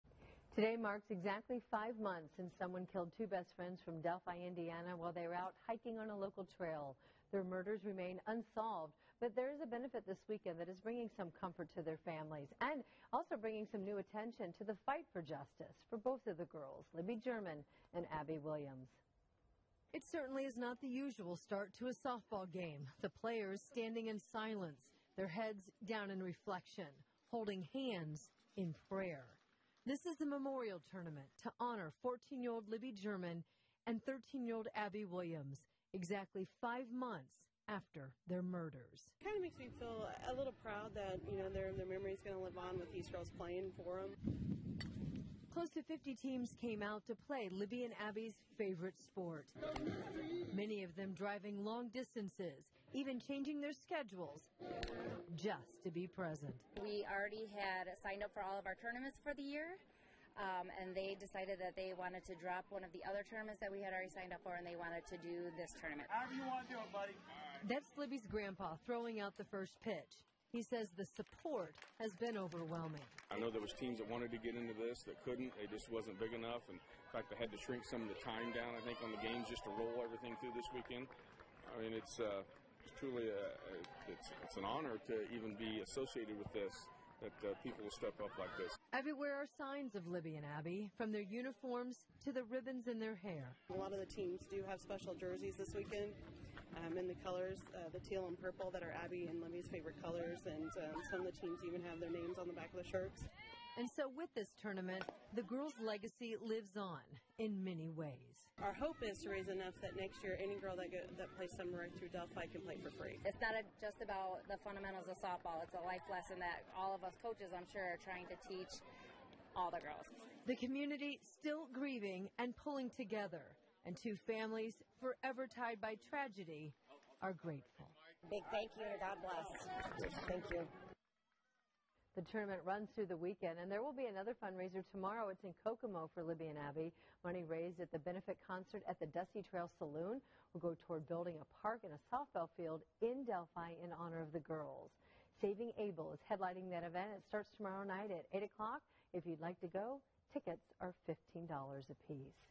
Delphi charity softball game
A special softball game was held Friday in memory of Libby German and Abby Williams.